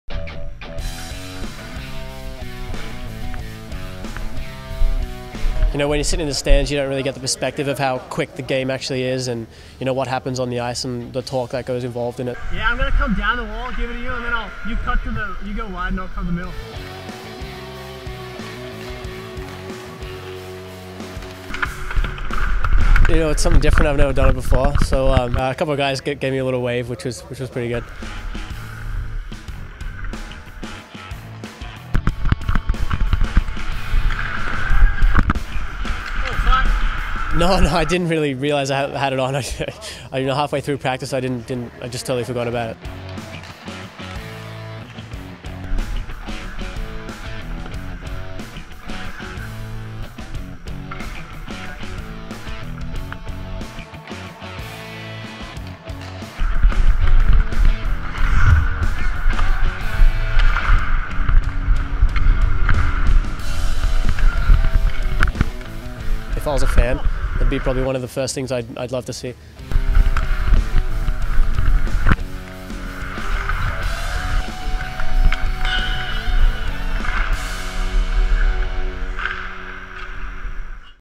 Nathan Walker's Helmet Cam at Caps Development Camp 7/9/14
Get Nathan Walker's perspective of Caps Development Camp with the view from his helmet camera.